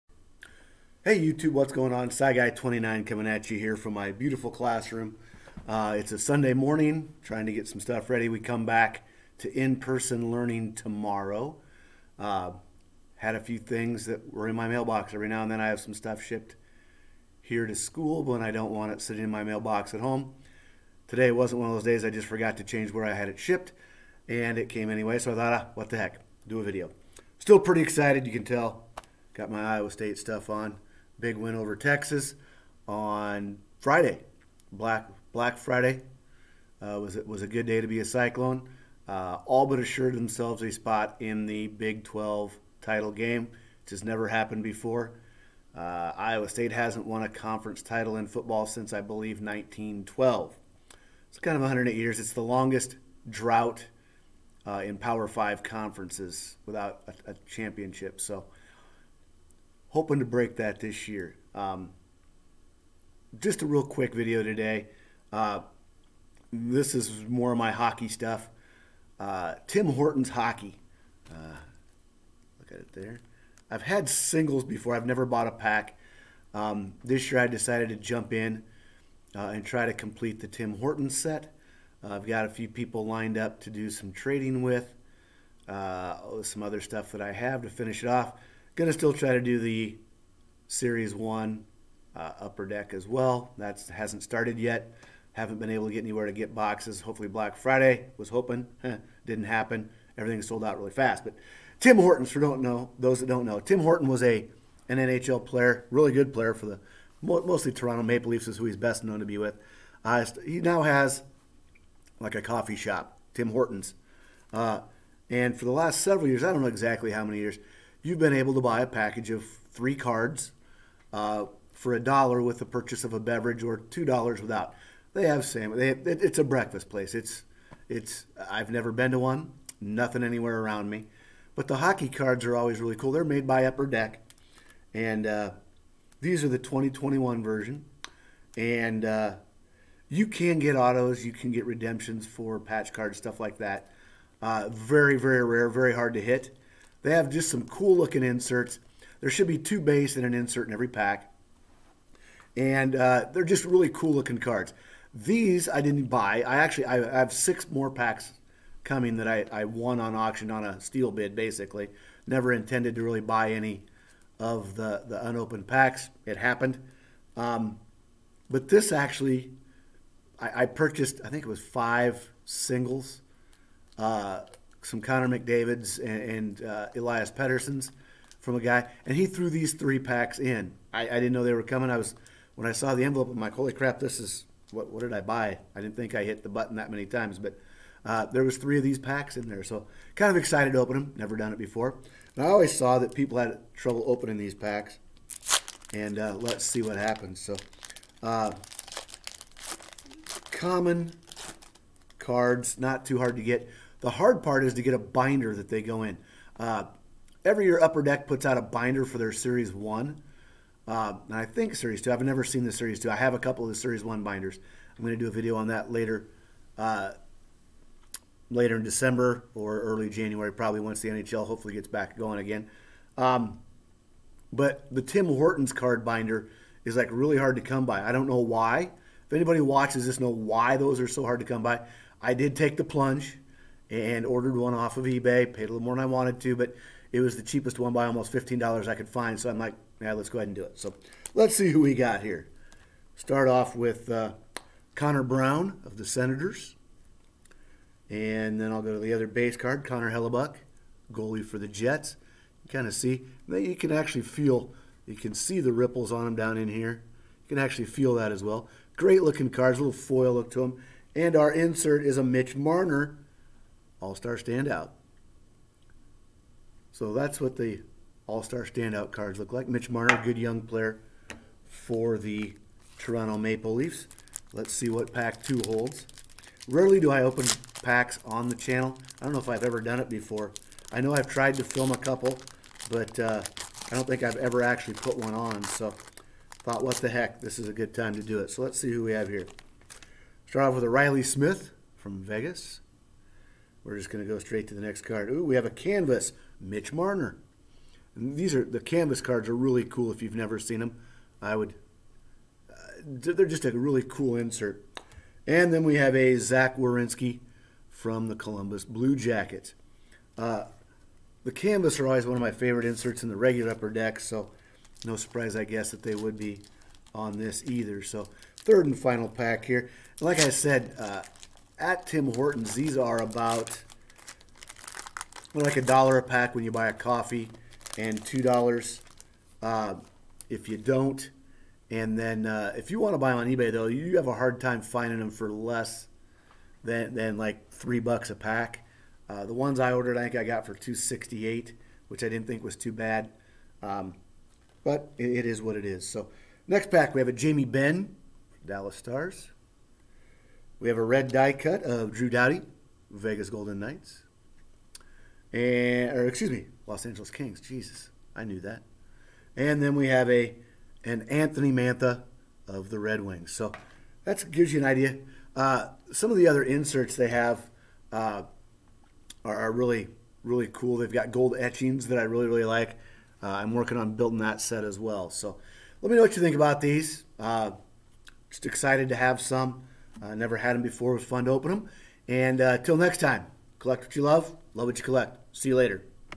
Surprise Gift!
Had a real surprise when I opened a purchase to find 3 unopened packs of Tim Hortons Hockey Cards.  Wasn't expecting it but greatly appreciate it.